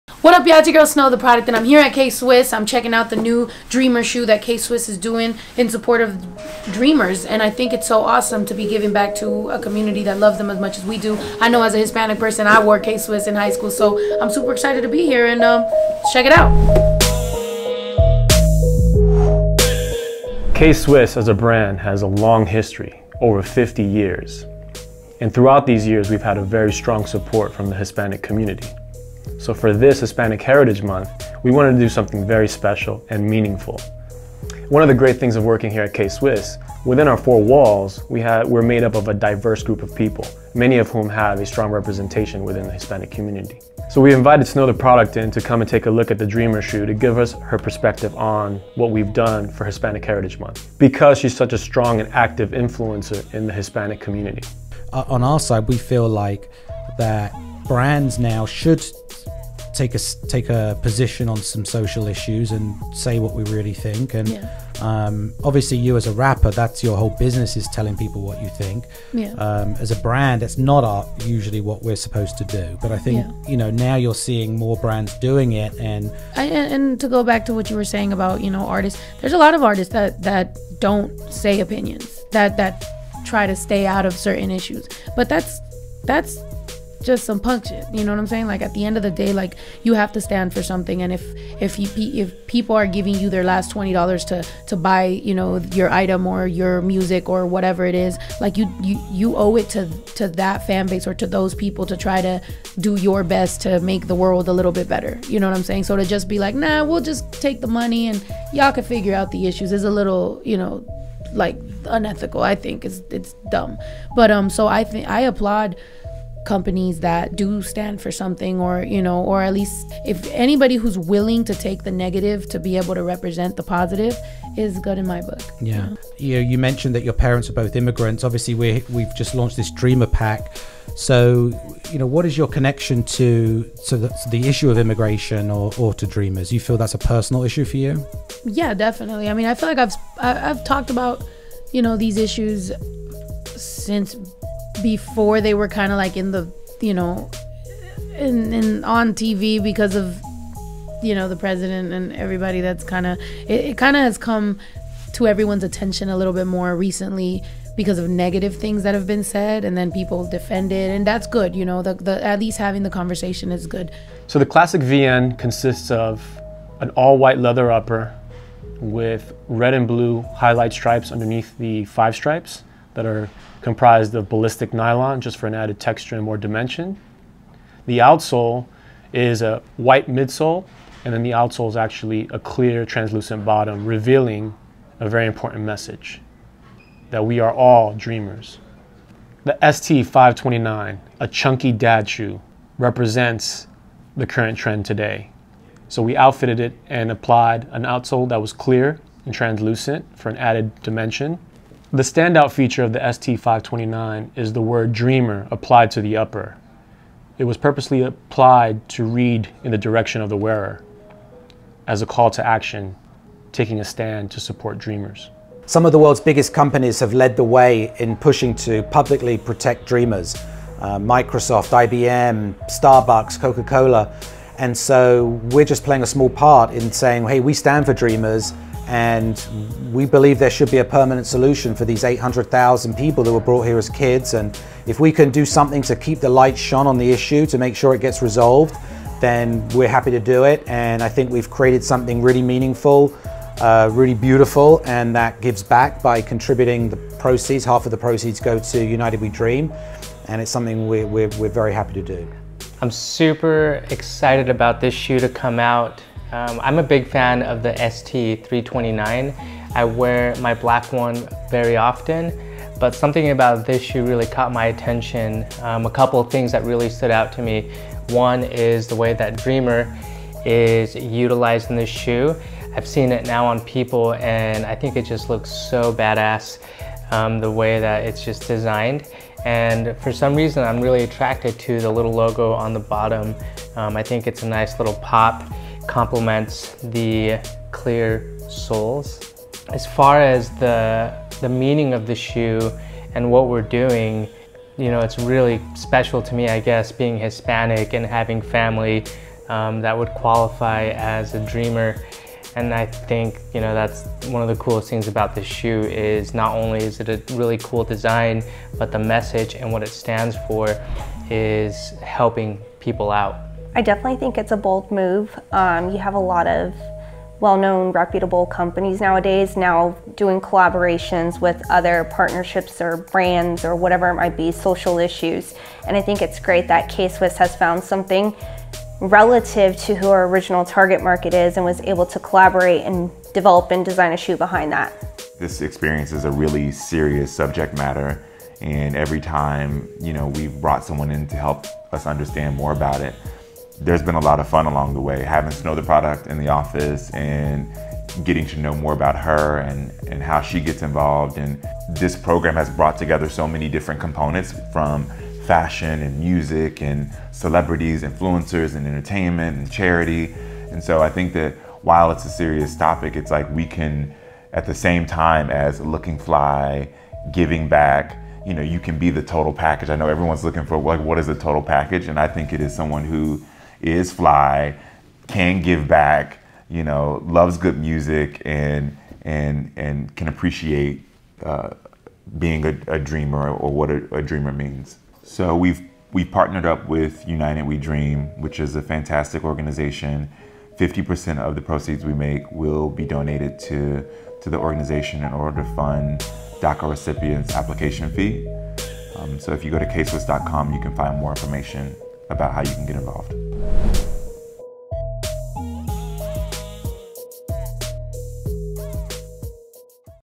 Inside K-Swiss Vlog Ep. 08 | Snow tha Product
In this episode we’re joined by rapper and entrepreneur, Snow tha Product, as we release our DREAMer Pack of sneakers.  With a combination of hustle, determination and will to succeed, Snow has built a brand that is both unique and influential, spanning music, acting and fashion.

Barney, Hommy and Snow discuss the importance of brands taking a stand for social justice issues. They talk about the DREAMer Movement and its relevance in today’s society.

Snow discusses her pride in her heritage, her entrepreneurial spirit, and gives advice for any dreamers out there who wish to make a difference.

To mark Hispanic Heritage Month we have released the DREAMer pack, to keep the spotlight on the need for a permanent solution for the 800,000 DREAMers in the US.

50% of proceeds generated by K-Swiss from the DREAMer pack will be donated to United We Dream to fund renewal fees for DACA recipients.

United We Dream is the largest immigrant youth-led organization in the nation. UWD organizes and advocates at the local and national levels for the dignity and justice of immigrants and communities of color in the United States.